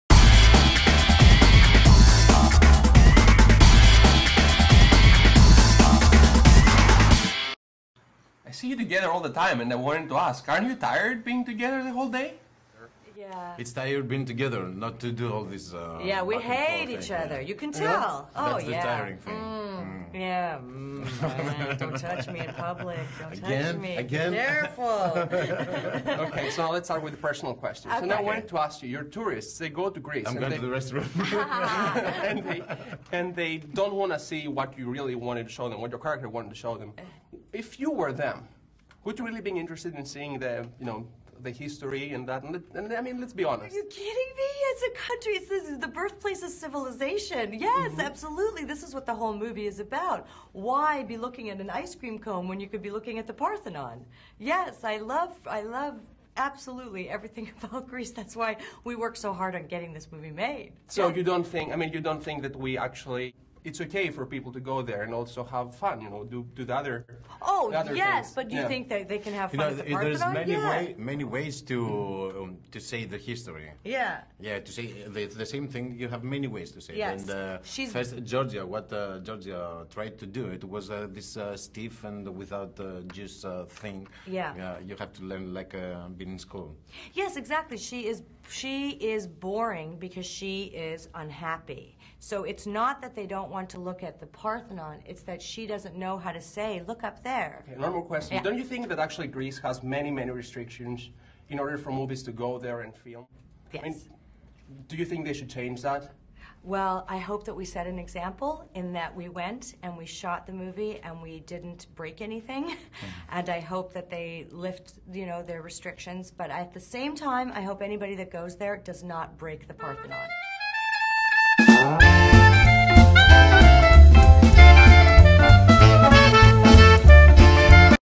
Nia Vardalos & Alexis Georgoulis talk about being a tourist and Filming in Greece 'My Life In Ruins'
We met Nia Vardalos and Alexis Georgoulis at the Press Day at the Four Seasons in Beverly Hills and talked about their new movie, being a tourist and the restrictions of filming in Greece and Parthenon.